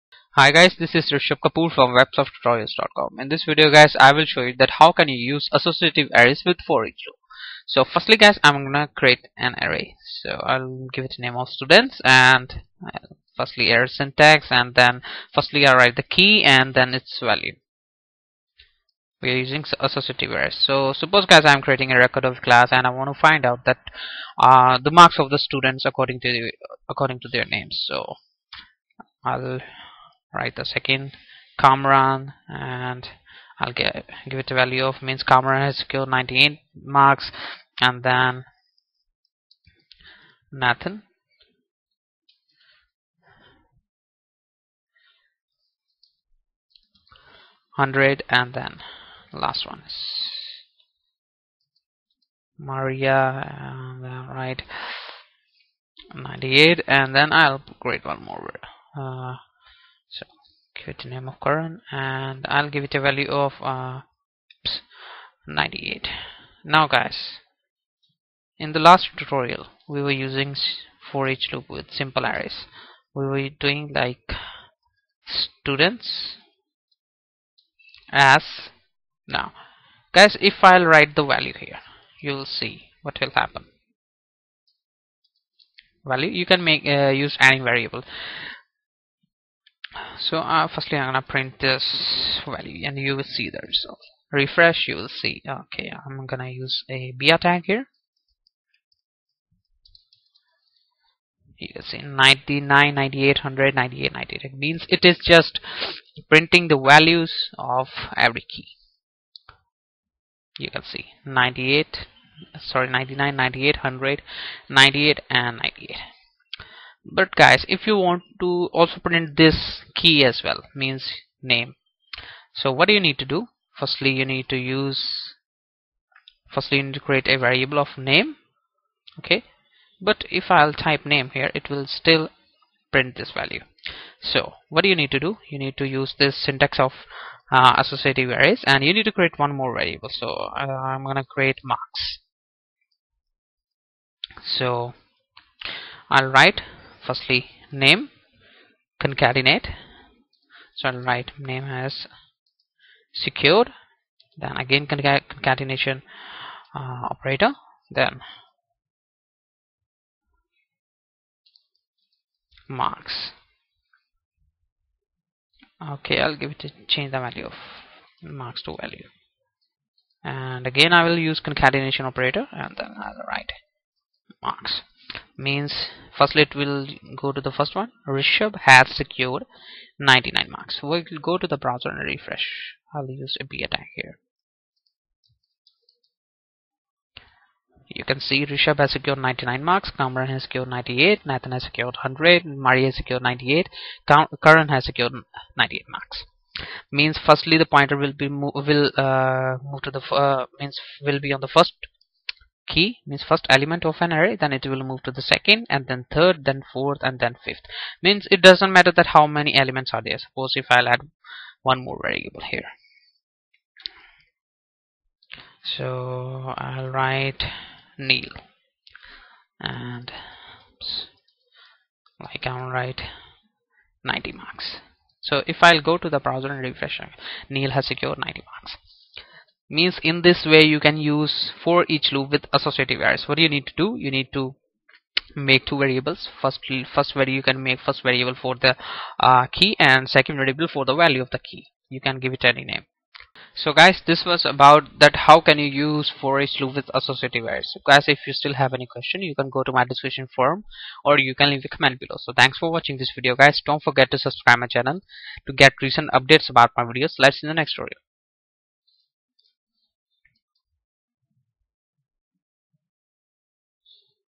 Php tutorials for beginners - 27 - how to use foreach loop with associative array
In this tutorial I will teach you how to use foreach loop with associative array